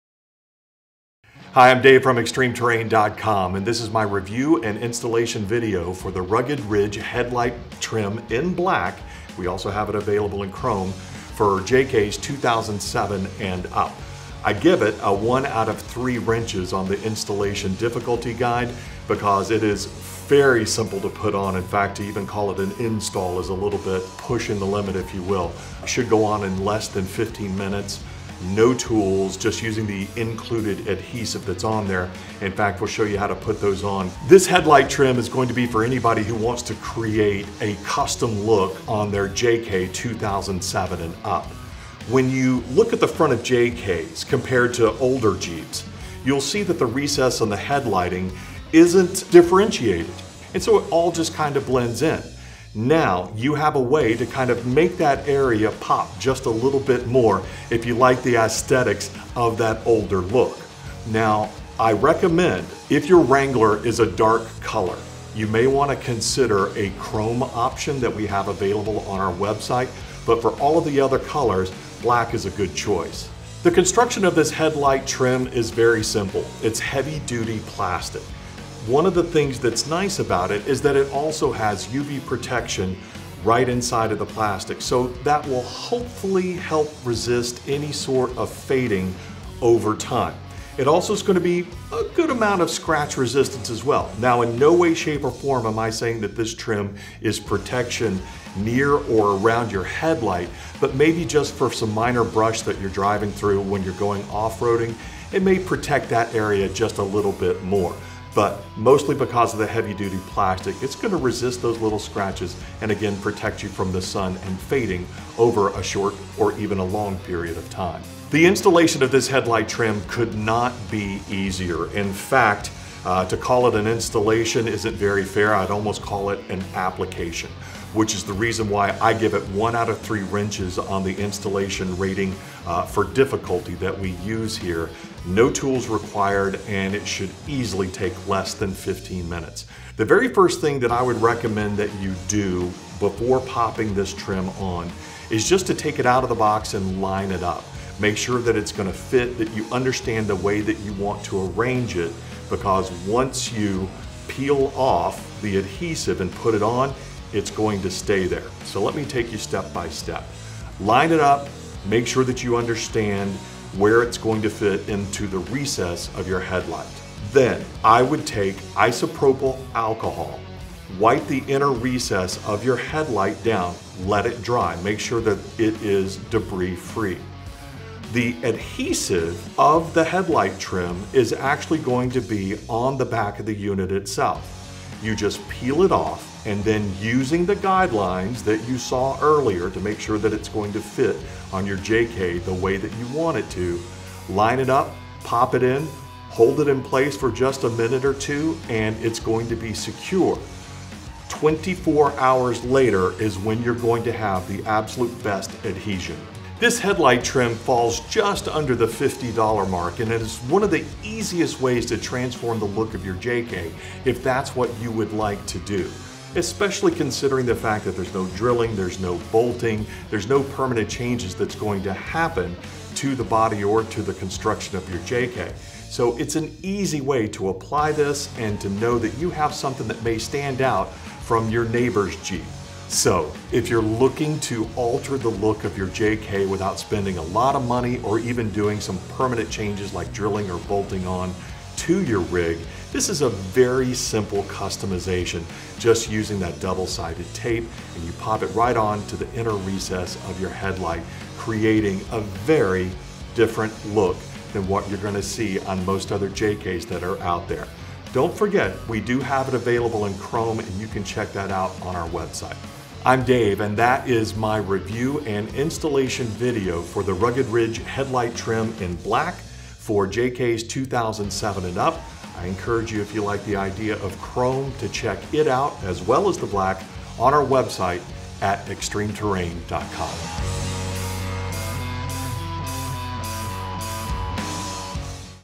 Jeep Wrangler Rugged Ridge Headlight Trim (2007-2017 JK) Review & Install
The Rugged Ridge Headlight Trim adds a distinguished look to the Wrangler! This installs easily for making any build stand out on the road or trails. Black Chrome

Black: Item J11182
MPN# 12419.25
Chrome: Item J12653
MPN# 13311.20

Unique Headlight Styling. The Rugged Ridge Black Headlight Trim will give your 2007 to 2017 Wrangler JK a unique and rugged style. This kit is a trim ring that fits effortlessly around the headlights of your Jeep. Designed to be scratch and fade resistant, the matte black finish of this headlight trim will blend well with the off-road theme of your rig. Perhaps the best part about this kit is the easy and hassle-free install with no drilling required. Looking for a way to differentiate the front fascia of your 2017 to 2017 Wrangler JK from other off-road machines? The Rugged Ridge Black Headlight Trim is an easy and inexpensive way to add unique styling to your Jeep.

Heavy Duty Construction. Made from heavy-duty plastic, the Rugged Ridge Black Headlight Trim is UV treated right out of the box to resist cracking, fading, and weathering even when constantly exposed to harsh weather elements. The matte black finish is also scratch resistant to maintain a lasting like-new appearance.

No Tools Neded. No drilling, no bondo, and no further modifications required. Simply clean the area around the headlights with alcohol to remove dust, dirt, and oil. Remove the protective backing on the 3M automotive bonding tape and press firmly to adhere.